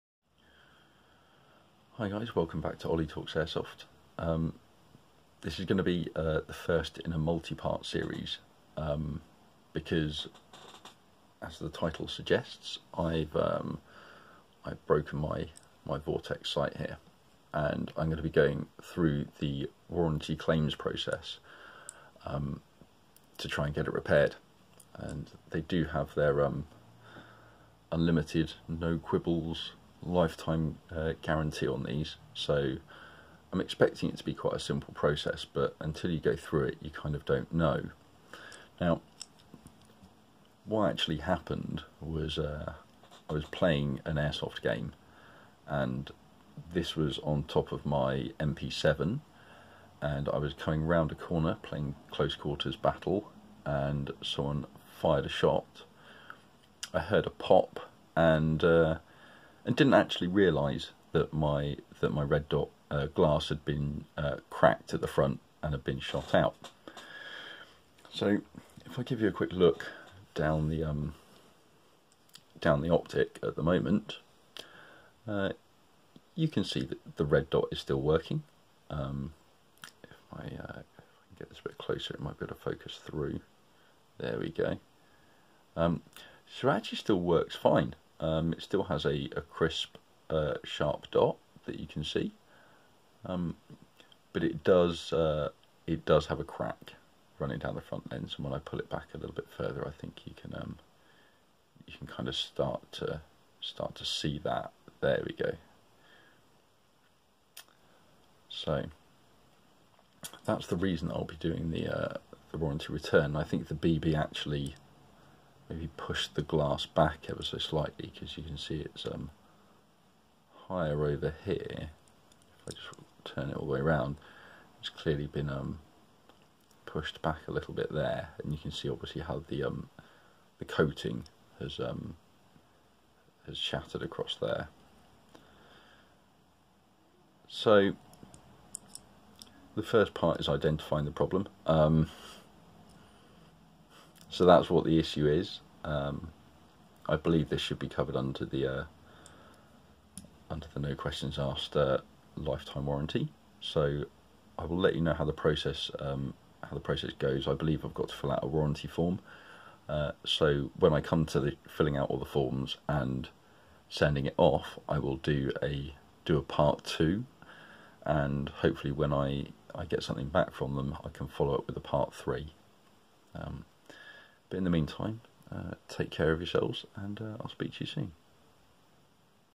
Vortex Crossfire Red Dot Cracked Lens VIP Warranty Return Part 1
so I got my front lens shot out of my vortex crossfire and need to return it, lets see how it goes with part 2 on the way